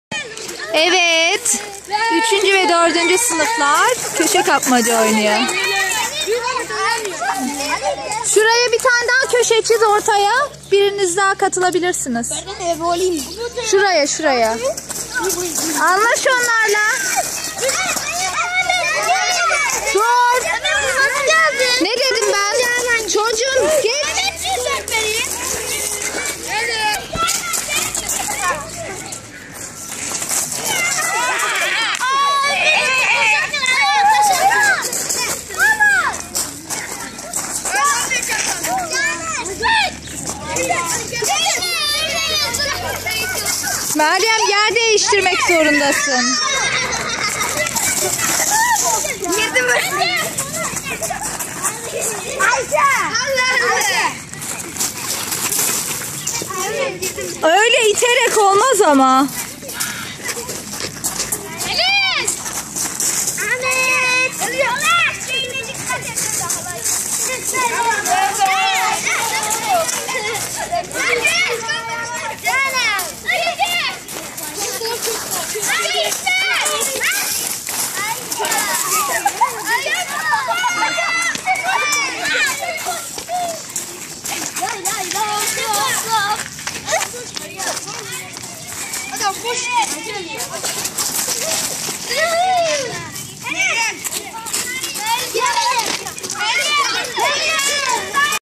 Aydınyurdu İlkokulu öğrencileri köşe kapmaca oynuyor :)